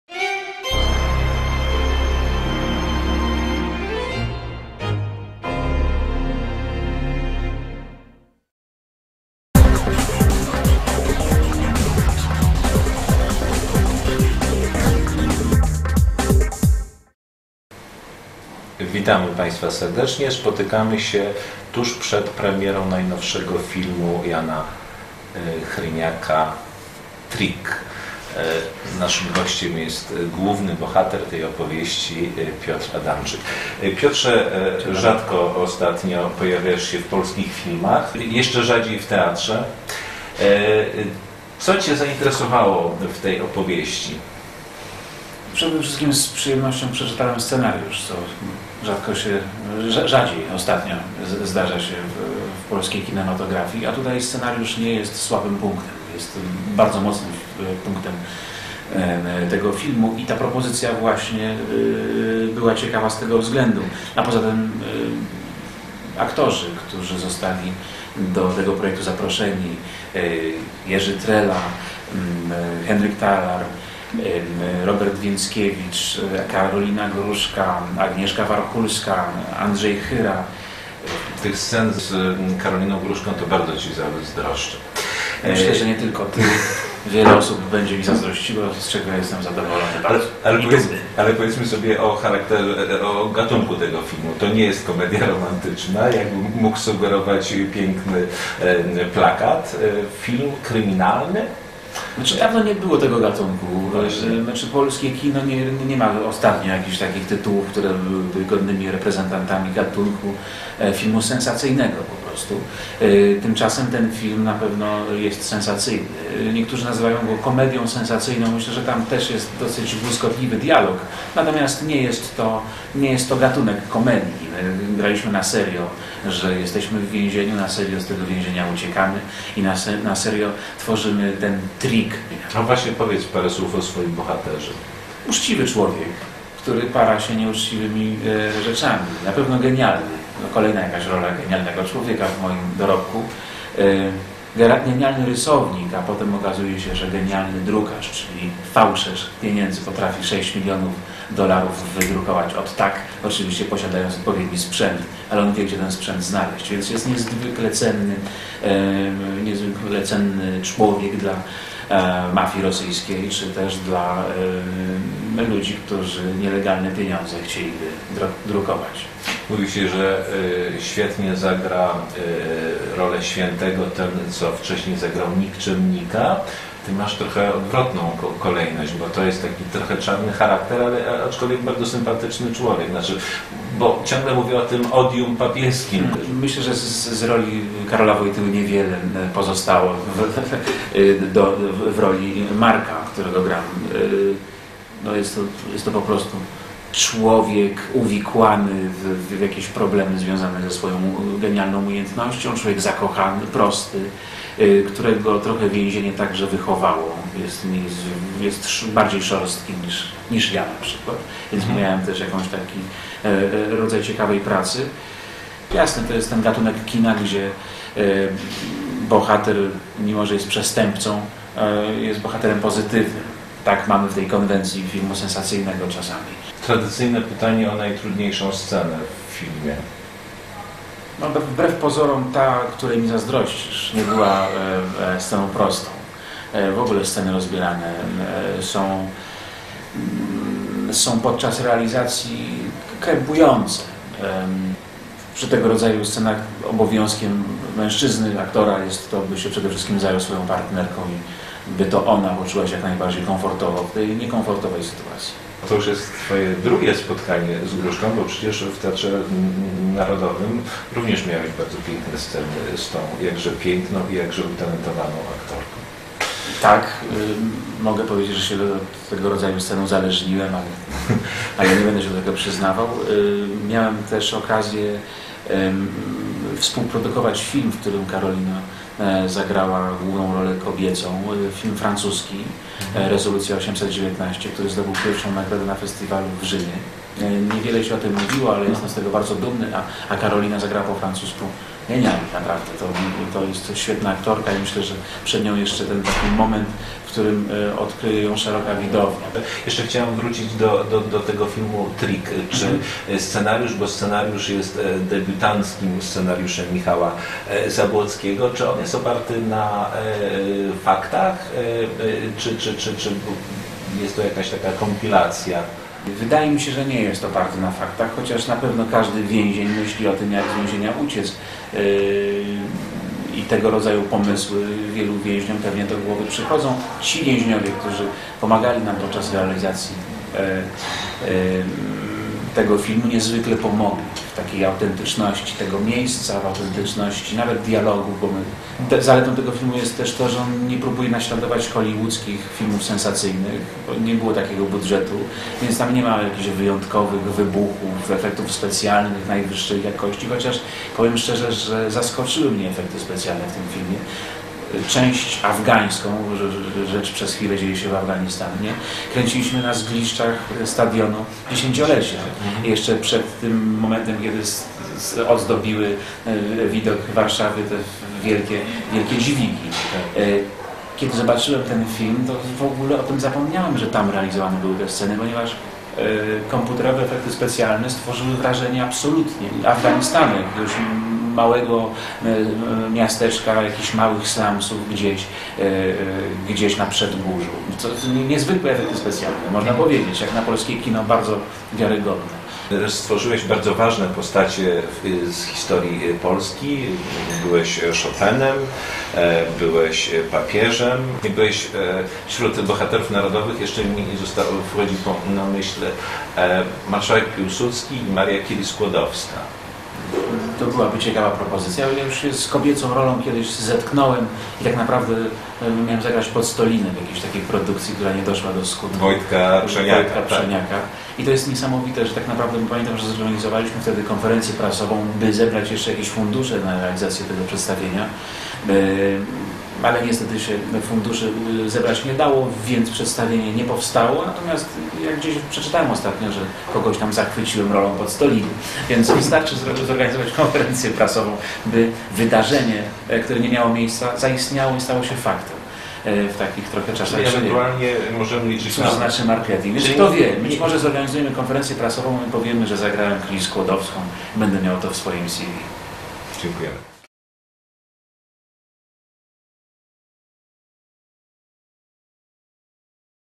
"Trick" Piotra Adamczyka
Jan Bończa-Szabłowski rozmawia (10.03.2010) z Piotrem Adamczykiem o filmie „Trick", który wszedł na ekrany polskich kin 12 marca. Z aktorem spotkaliśmy się w Teatrze Narodowym w Warszawie.